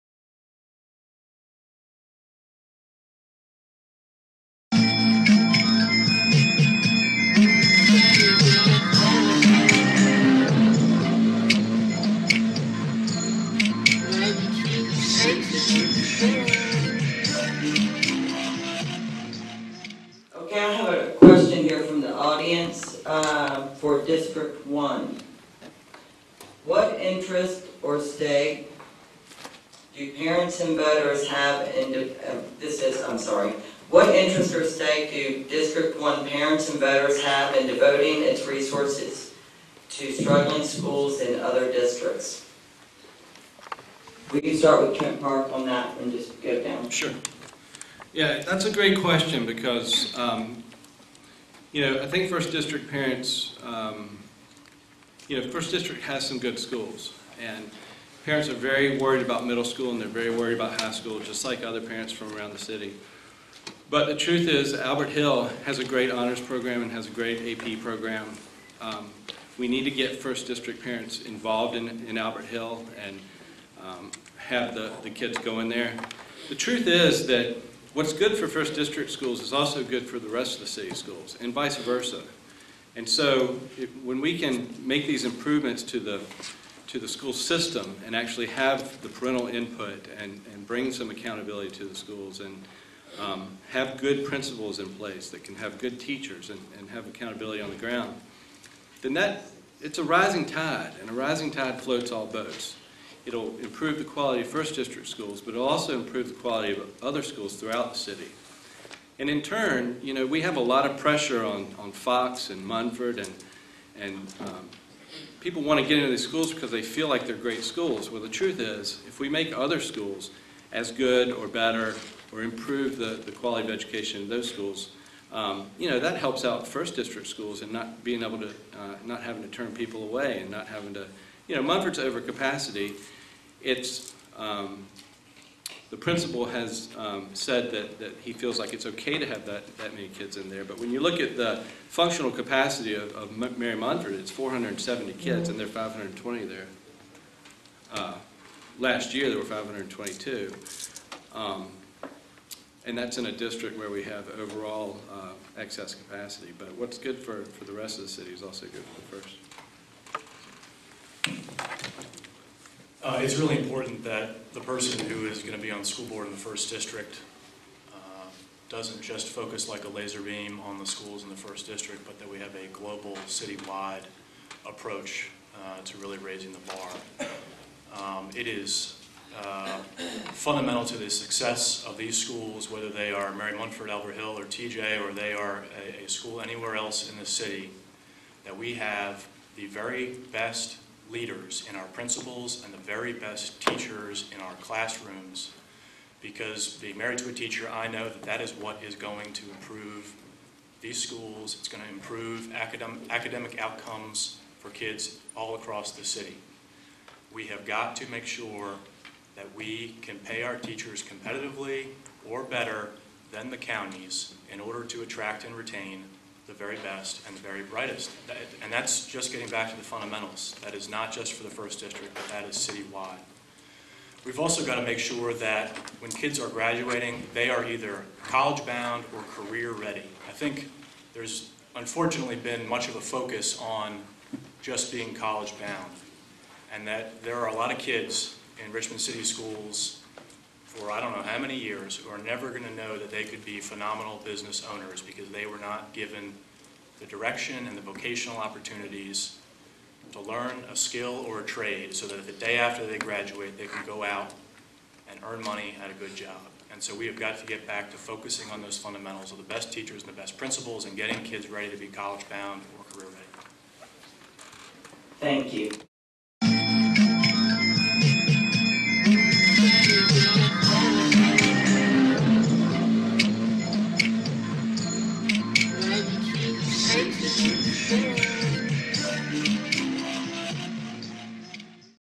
Part 11/15 School Board Candidate Forum - 2012 Sept 20 - 1st District's Interests - Richmond, VA
What interest or stake do 1st District Parents and Voters have in devoting its resources to struggling schools in other districts?

First District candidates Trent Park [0:47] and Glen Sturtevant [2:41] responded.

The event was organized by the VCU Political Science Department, the A. Philip Randolph Institute, The Alliance for Progressive Values, The Coalition to Stop Gun Violence, People of Faith for Equality, RePHRAME - Residents of Public Housing in Richmond Against Mass Evictions, Richmond NOW, Sierra Club - Fall of the James, S.O.N.G. Southerners On New Ground, UniteWomen.org - Virginia Chapter, Virginia New Majority - Central Virginia Chapter, and Virginia Organizing.